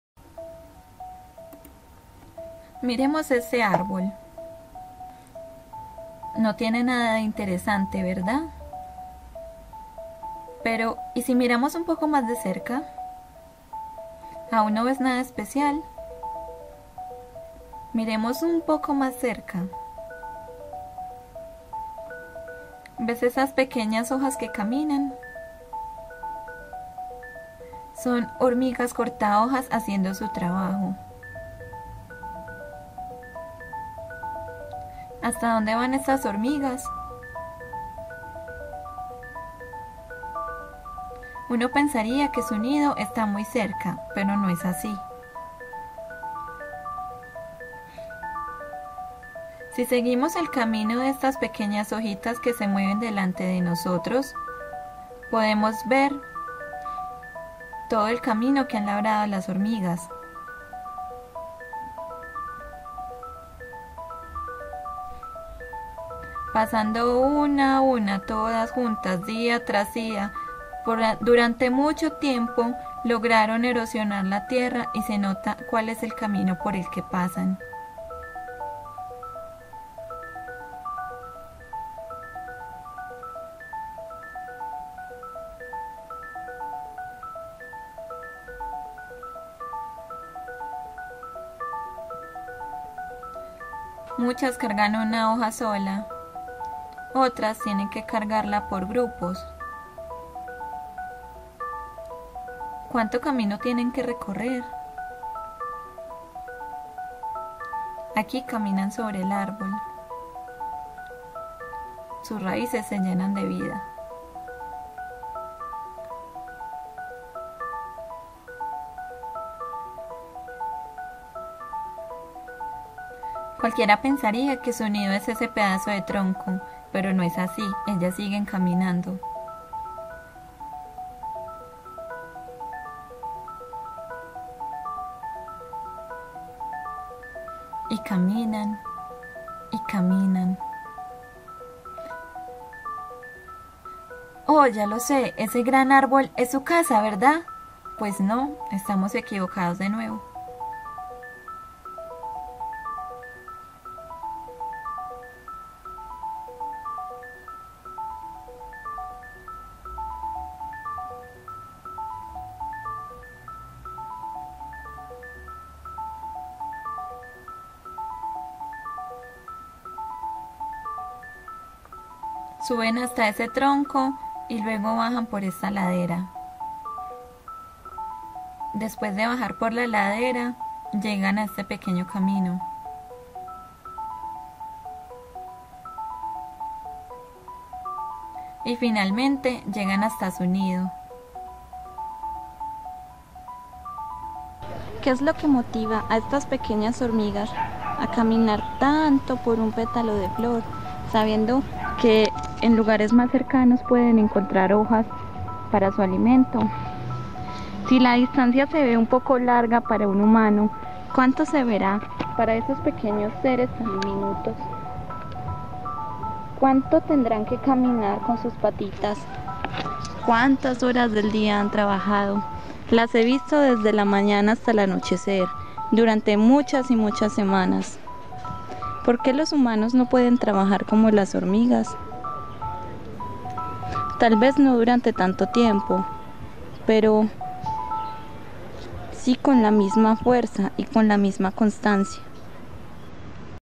el trabajo de las hormigas (pequeña vicky reflexión)
Me dio un momento de reflexión cuando vi estas hermosas hormigas trabajando. Espero que les guste. Ya saben trabajemos así sea con la constancia y compañerismo de las hormigas y conseguiremos muchas cosas en la vida.